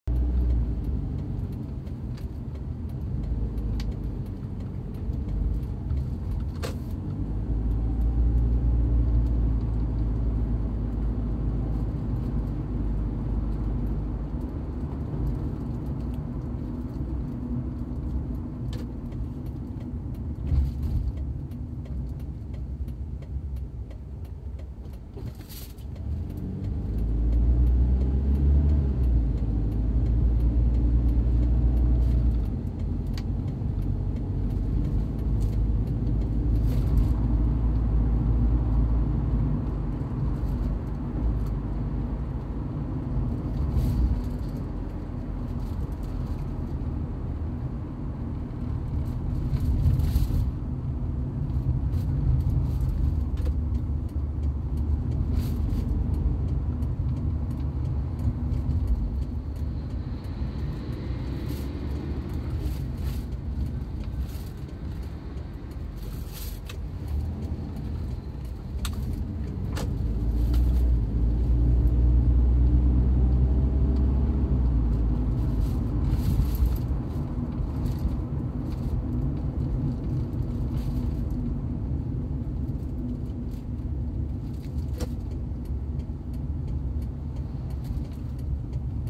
ウェザーリポートナビ動画0709@神宮球場～国立競技場建設予定地。昨日、8日の15:13～15:15 です。
昨日、動画あげられなかったので9日の朝7:00にあげました。